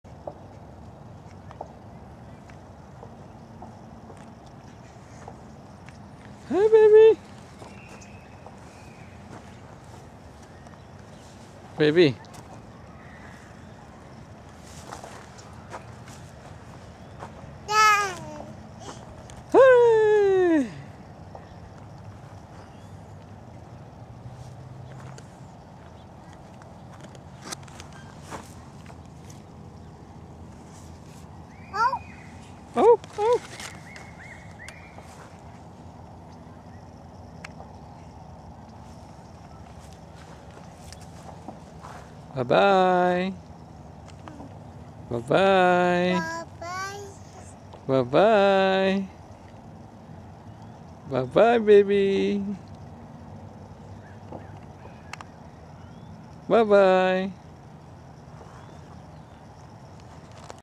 1st day at the park
my baby 1st day at the park...feel lonely boring...